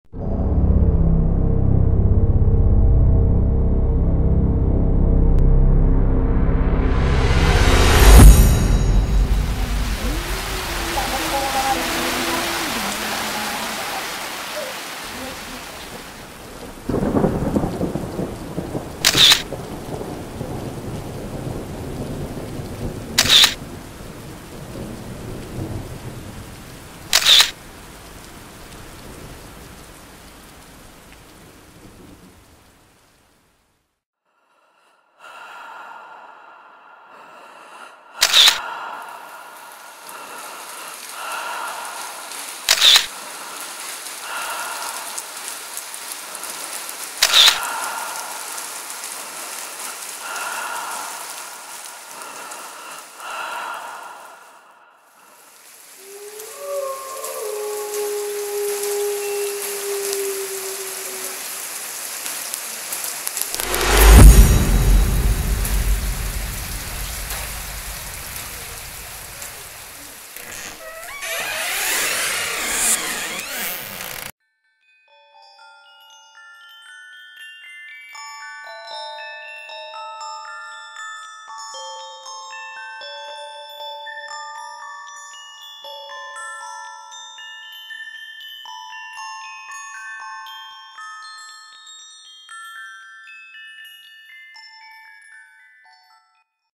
BELASAH trailer
iseng terjebak hujan tengah malam, numpang berteduh...
drpd bengong... nge_take lahh
jgn tanya kawalitas gambar, pst ancor..
cuma pake HP jadul S6310..
... jgn serius kali juga ...hehehe